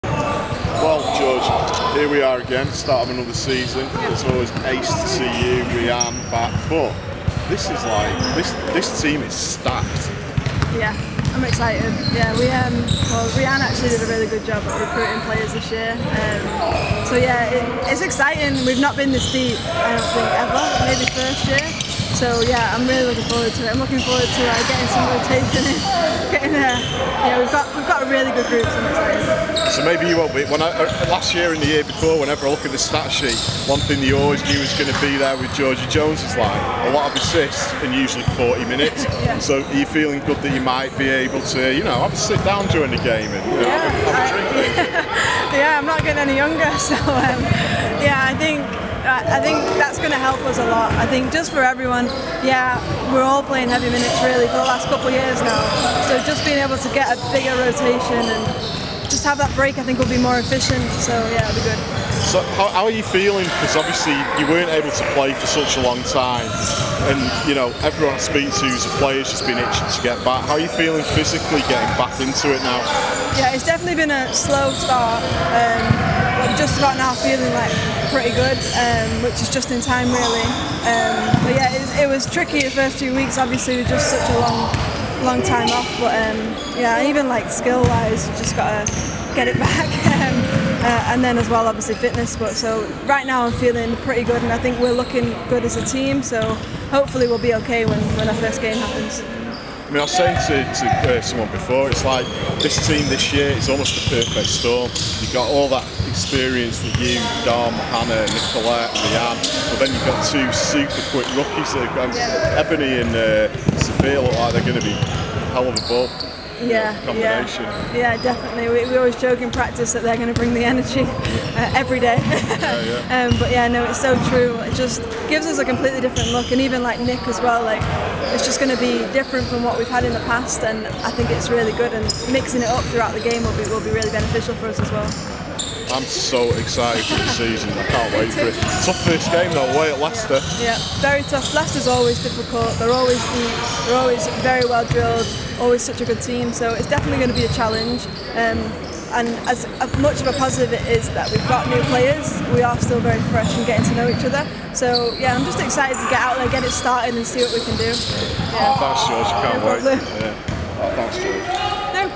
Georgia Jones- Manchester Mystics WBBL & GB Basketball Pre season 20/21 Interview
I caught up with Georgia Jones at practice to talk about the new look Mystics! The veteran Point Guard begins the season as WBBL all time assists leader!